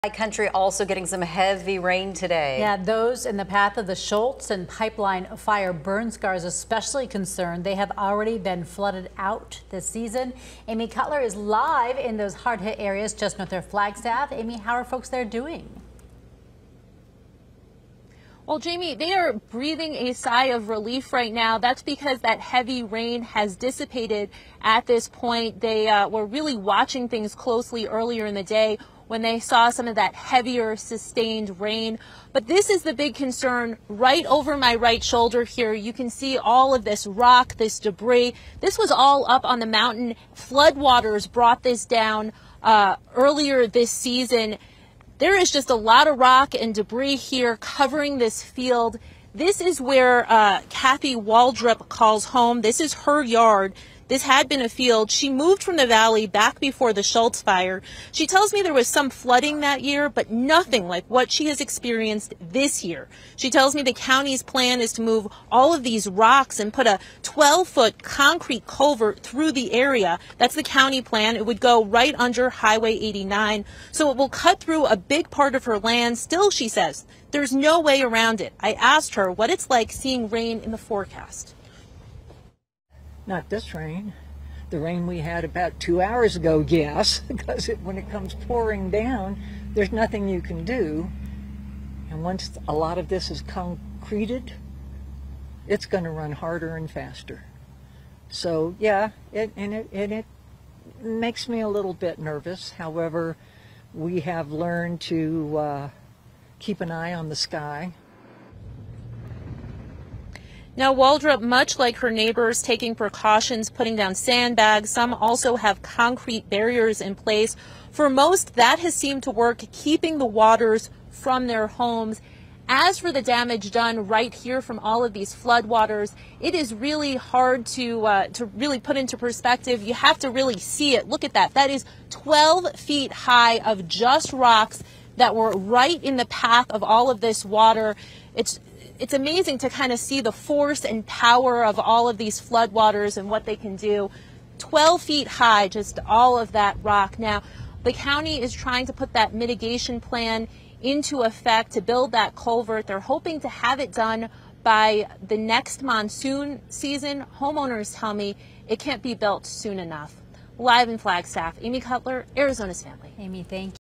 Residents near Flagstaff burn scars preparing for storms
Homeowners saw rain overnight and into Wednesday morning, growing concerned about more flooding near their homes.